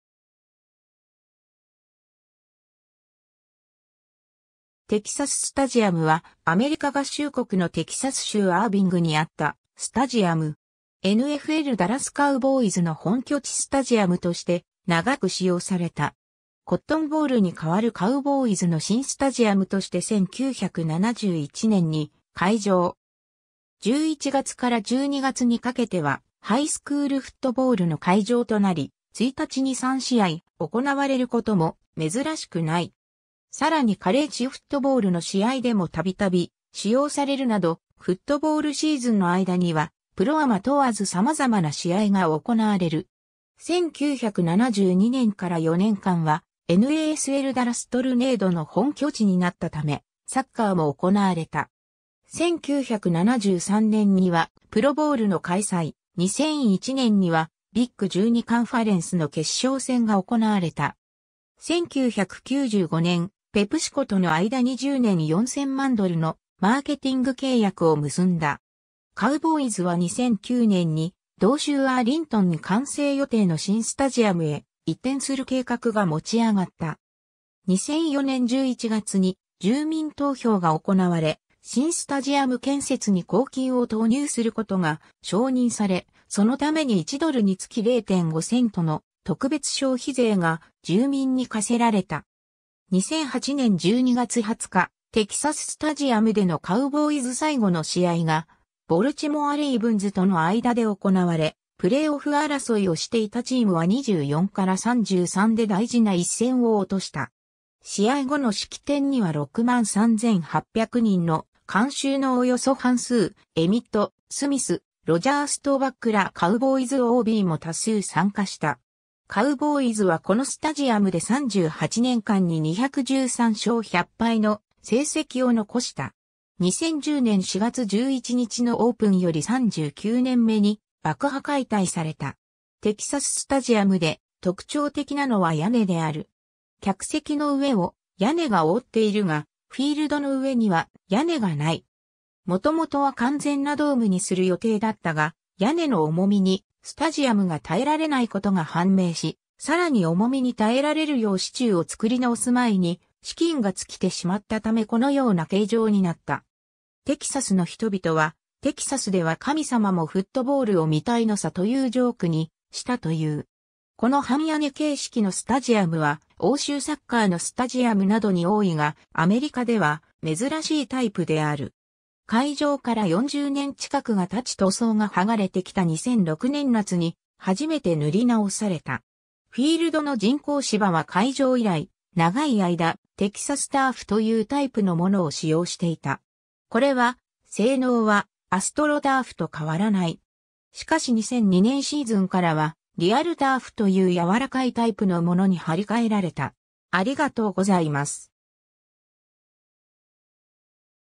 テキサス・スタジアム
ダラス・カウボーイズ
アービング_(テキサス州)
テキサス・スタジアム（Texas Stadium）は、アメリカ合衆国のテキサス州アーヴィングにあったスタジアム。
NFLダラス・カウボーイズの本拠地スタジアムとして長く使用された。
 コットン・ボウルに代わるカウボーイズの新スタジアムとして1971年に開場。
11月から12月にかけてはハイスクールフットボールの会場となり、1日に3試合行われることも珍しくない。
さらにカレッジフットボールの試合でもたびたび使用されるなど、フットボールシーズンの間にはプロ・アマ問わず様々な試合が行われる。
1972年から4年間はNASLダラス・トルネードの本拠地になったため、サッカーも行われた。
 1973年にはプロボウルの開催、2001年にはビッグ12カンファレンスの決勝戦が行われた。
 1995年、ペプシコとの間に10年4000万ドルのマーケティング契約を結んだ。
 カウボーイズは2009年に同州アーリントンに完成予定の新スタジアムへ移転する計画が持ち上がった。
2004年11月に住民投票が行われ、新スタジアム建設に公金を投入することが承認され、そのために1ドルにつき0.5セントの特別消費税が住民に課せられた。
 2008年12月20日、テキサス・スタジアムでのカウボーイズ最後の試合がボルチモア・レイブンズとの間で行われプレーオフ争いをしていたチームは24-33で大事な一戦を落とした。
試合後の式典には63,800人の観衆のおよそ半数、エミット・スミス、ロジャー・ストーバックらカウボーイズOBも多数参加した。
カウボーイズはこのスタジアムで38年間に213勝100敗の成績を残した。
 2010年4月11日のオープンより39年目に爆破解体された。
 テキサス・スタジアムで特徴的なのは屋根である。
客席の上を屋根が覆っているが、フィールドの上には屋根がない。
元々は完全なドームにする予定だったが、屋根の重みにスタジアムが耐えられないことが判明し、さらに重みに耐えられるよう支柱を造り直す前に資金が尽きてしまったためこのような形状になった。
テキサスの人々は「テキサスでは神様もフットボールを見たいのさ」というジョークにしたという。
 この半屋根形式のスタジアムは欧州サッカーのスタジアムなどに多いが、アメリカでは珍しいタイプである。
開場から40年近くがたち塗装が剥がれてきた2006年夏に初めて塗り直された。
 フィールドの人工芝は開場以来長い間「テキサス・ターフ」というタイプのものを使用していた。
これは性能はアストロターフと変わらない。
しかし2002年シーズンからは「リアルターフ」という柔らかいタイプのものに貼り替えられた。